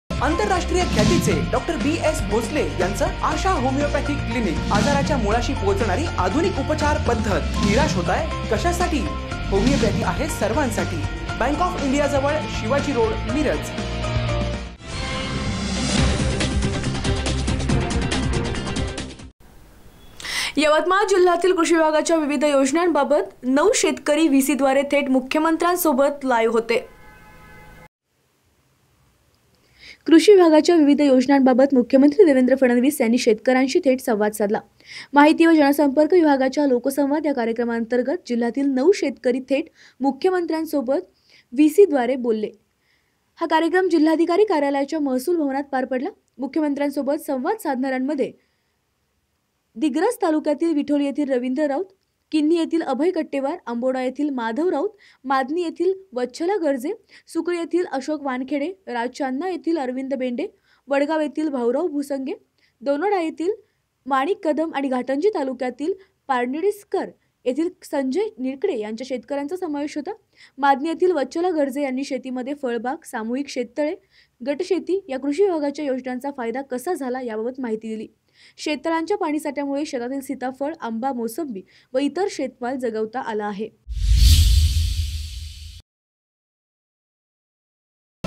यवतमाळ जिल्ह्यातील कृषी विभागाच्या विविध योजनांबाबत नऊ शेतकरी
यवतमाळ जिल्ह्यातील कृषी विभागाच्या विविध योजनांबाबत नऊ शेतकरी व्हीसीद्वारे थेट मुखमंत्र्यासोबत लाईव्ह.@